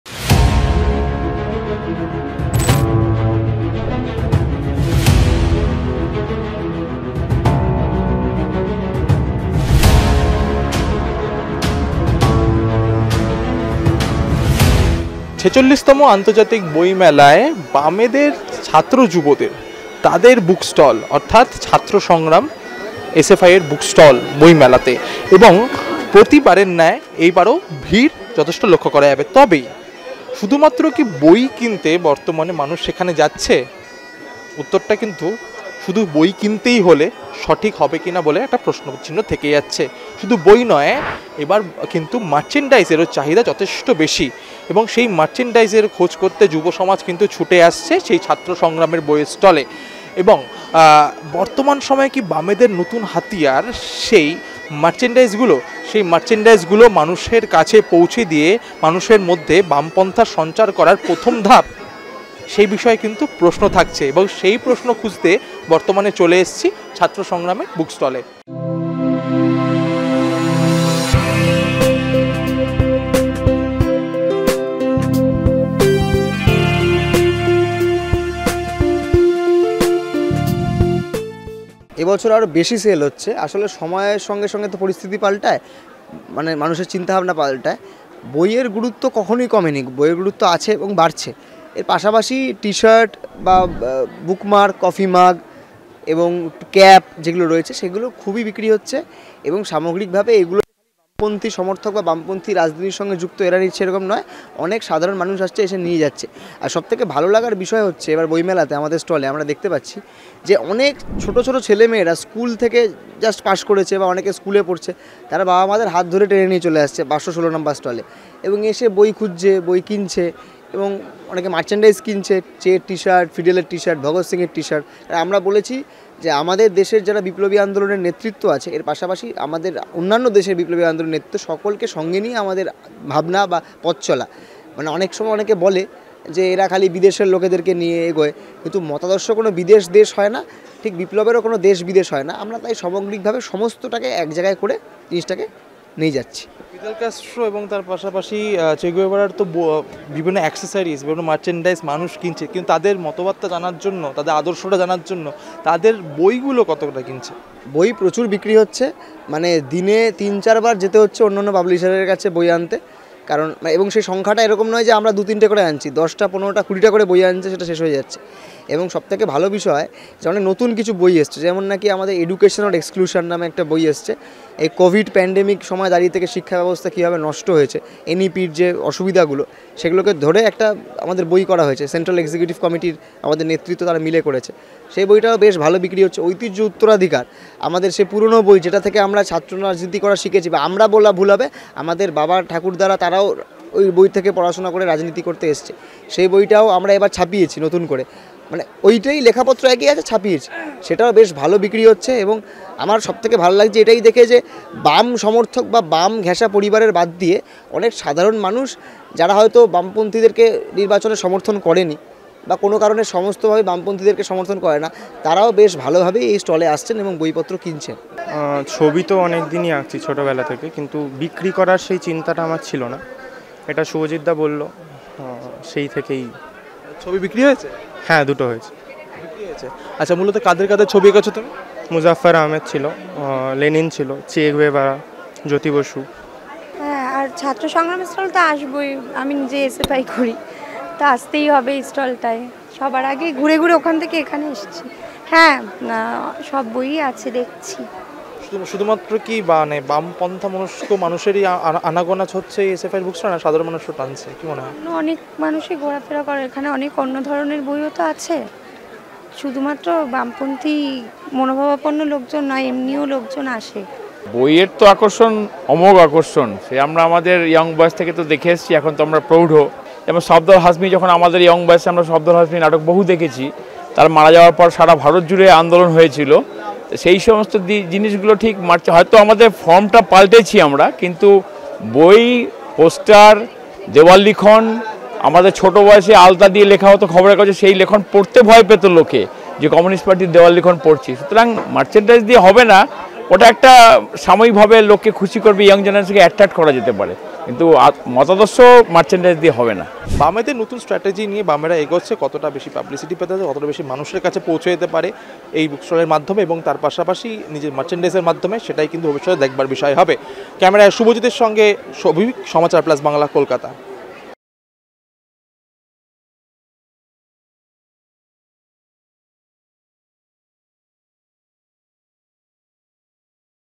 Kolkata Book Fair 2023 : বইমেলা কাঁপাচ্ছে ছাত্র সংগ্রাম স্টলের মার্চেন্ডাইজ ?
Kolkata Book Fair 2023 : বইমেলা কাঁপাচ্ছে ছাত্র সংগ্রাম স্টলের মার্চেন্ডাইজ ? বইয়ের থেকে এগিয়ে মার্চেন্ডাইজ বিক্রয় ?